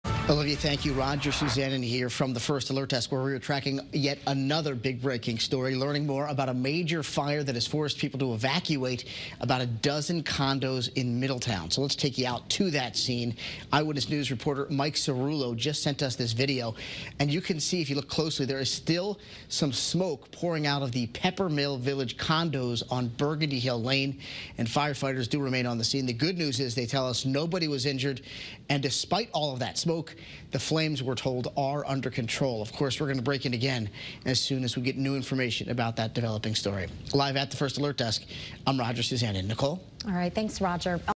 BREAKING: Early morning fire forces evacuations in Middletown
Firefighters battled an early morning fire in Middletown.